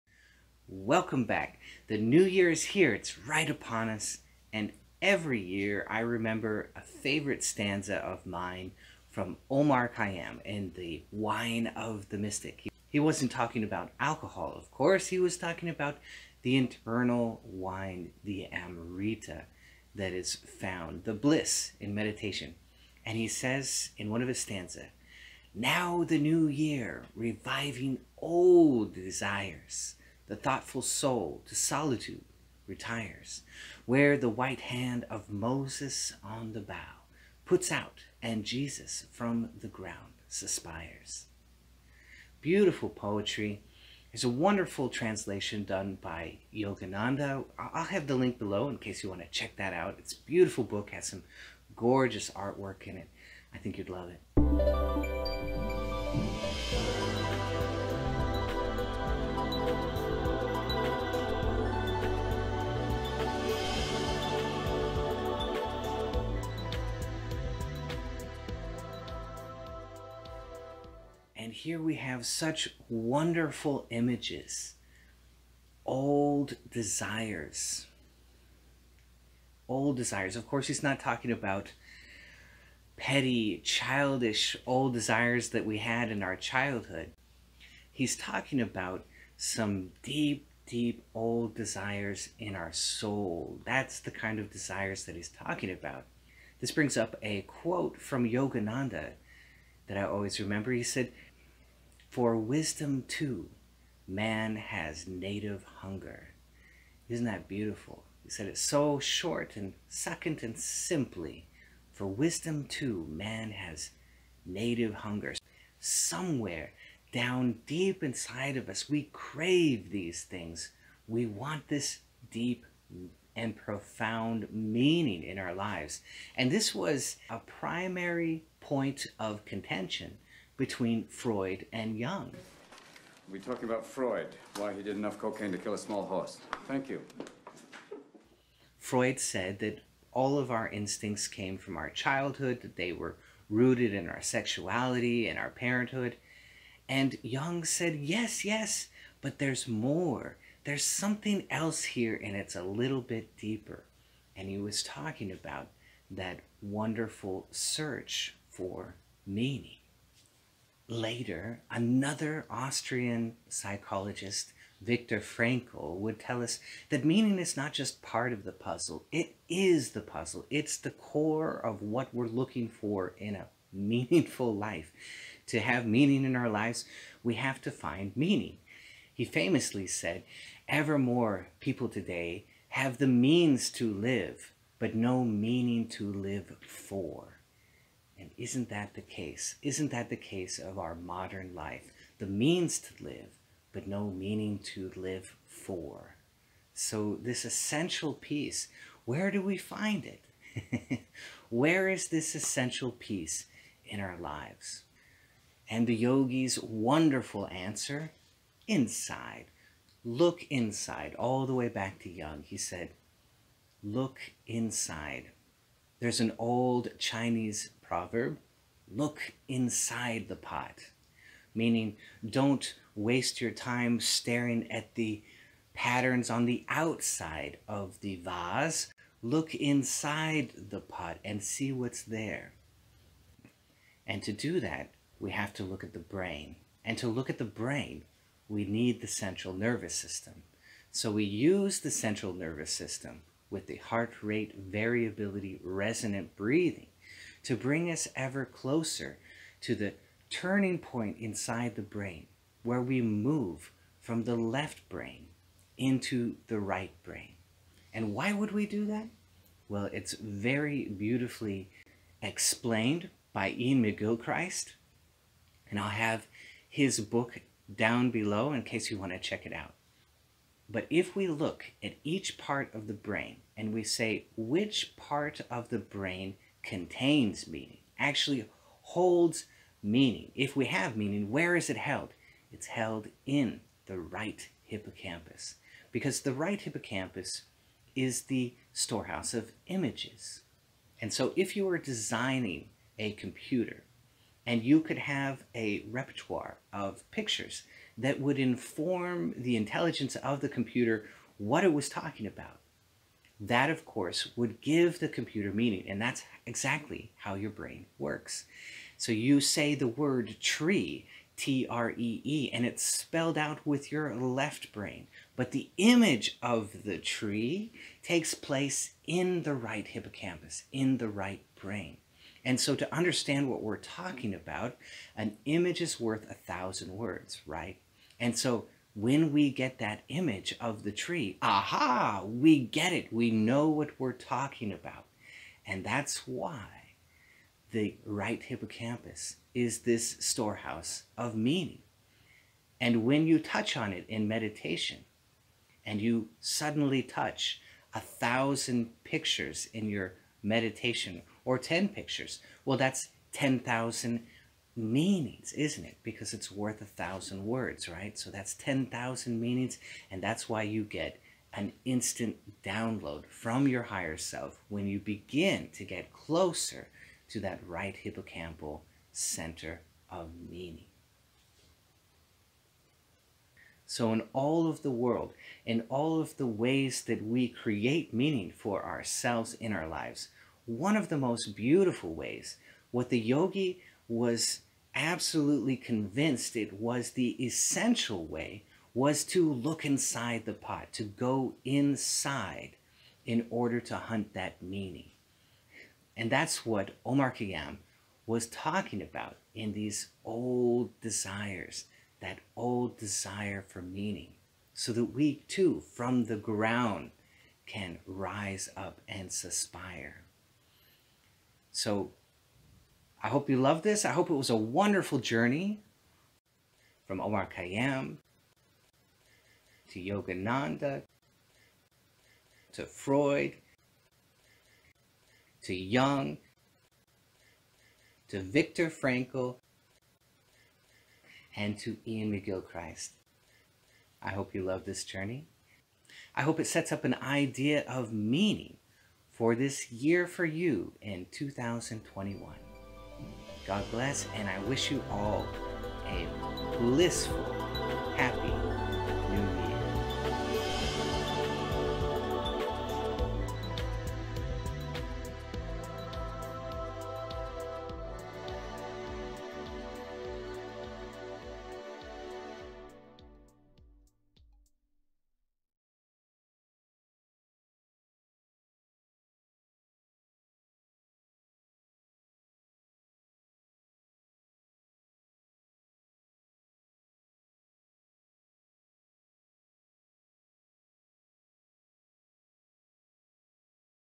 The Great Pursuit of Meaning
What exactly is the Great Quest that fills men's hearts and a yogi's internal life?

Omar Khayyam:
Wine of the Mystic

Iain McGilchrist books:
The Divided Brain
Ways of Attending
The Master and His Emissary

Viktor Frankl:
Man's Search For Meaning

Carl Jung:
The Archetypes & Collective Unconscious

Thank you to Doug Gemmell for the amazing music!